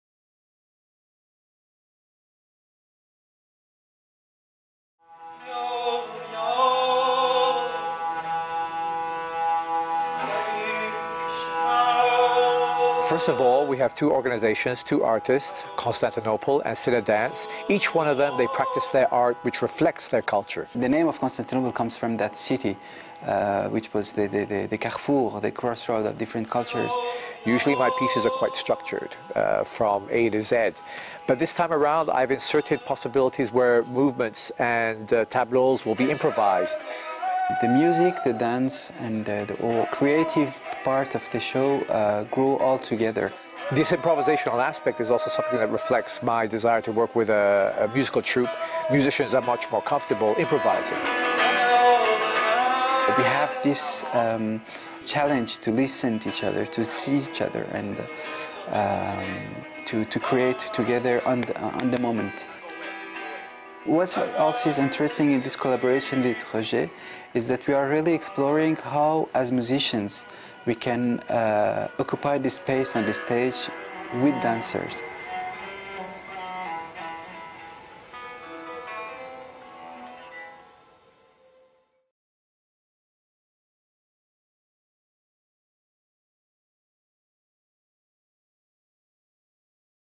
Śūnya | Sinha Danse
Śūnya, 65 min

«C'est la pièce la plus réussie de Roger Sinha... La collaboration artistique avec Constantinople fonctionne à merveille...» - Frédérique Doyon, Le Devoir

«À la fois extrêmement poétique, inspiré et inspirant, hypnotique et émouvant. Une pièce sublime, une invitation à un voyage subtil aussi sensuel que spirituel, … d’une beauté émouvante.» - Aline Apostolska, La Presse

«Une ambiance envoutante. Un spectacle fabuleux et particulièrement beau... » - Marie-Christine Trottier, Bouillant de culture, Radio Canada

«...Sunya est une collaboration unique et totalement réussie...» - Nancy Berman, Bachtrack

«Cette création surprend et émeut par la profondeur de la démarche et la grande sensibilité qui s’en dégage. Sunya constitue sans contredit une œuvre forte, qui interpelle véritablement.» - Laure Ghelfi, PatWhite

Tout son résulte d'un geste, mais dans l'abyme du silence, le mouvement pourrait-il éclore ? Toute expression s'élabore à partir d'une mémoire, mais lorsqu'elle est plurielle, quelle est sa résonance ? Et que se passe-t-il lorsque deux créateurs, l'un chorégraphe, l'autre compositeur, chacun à la croisée des cultures, entrent en connivence à partir de rien, à partir de tout ce qui les constitue ?

Śūnya (en sanscrit, «zéro» et «chiffre») désigne ce paradoxe fondateur de l'être, du langage, du mouvement... Mouvement de l'exil, mouvement de l'art et du métissage.
Quatre danseurs, trois musiciens et un vidéaste expérimentent au présent, tour à tour guidés par le chorégraphe indo-arménien Roger Sinha et par le découvreur aux racines iraniennes Kiya Tabassian, tous deux ivres de dialogue.

De l'universelle quête identitaire à l'épopée collective, un geste de création comme l'écho onirique d'un monde réconcilié.

Roger Sinha (Sinha Danse) et Kiya Tabassian (Constantinople) unissent leur immense talent dans Śūnya, une nouvelle création. Rencontre originale entre la danse contemporaine et la musique d’inspiration persane, Śūnya explore, entre autres, le thème du métissage des cultures. Roger Sinha s’intéresse depuis toujours à l’immigrant, à « l’autre », au déraciné. Il était donc tout naturel qu’il s’associe à l’ensemble Constantinople, passé maître dans le dialogue entre musiques anciennes, de la Méditerranée et de l’Orient, pour évoquer les défis des rencontres culturelles.

Année de création: 2012-13
Chorégraphe: Roger Sinha, en collaboration avec les interprètes
Interprètes:
Danseurs : Thomas Casey, Tanya Crowder, Ghislaine Doté, François Richard
Musiciens : Kiya Tabassian (sétar et voix), Ziya Tabassian (percussions), Pierre-Yves Martel (viole de gambe)
Conception musicale : Kiya Tabassian en collaboration avec Ziya Tabassian, Pierre-Yves Martel
Direction technique et conception lumières : Caroline Nadeau; Conception visuelle et design d'interactions : Jérôme Delapierre; Sonorisation: Pablo Bonacina; Dramaturgie : Jo Leslie; Directrice de répétition : Sarah Williams
Direction artistique: Roger Sinha et Kiya Tabassian en collaboration avec Jo Leslie